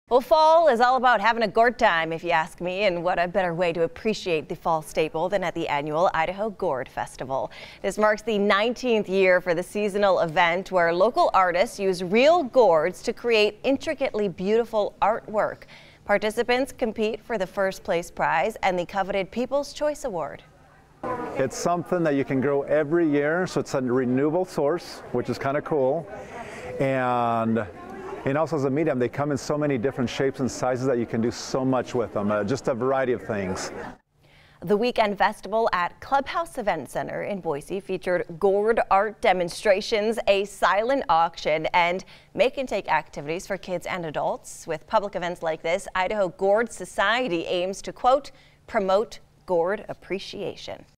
Local artists use real gourds to create intricately beautiful artwork
This weekend marks the 19th year for the Idaho Gourd Festival, where local artists use real gourds to create intricately beautiful artwork.  Participants compete for the first place ribbon and the coveted "People's Choice Award."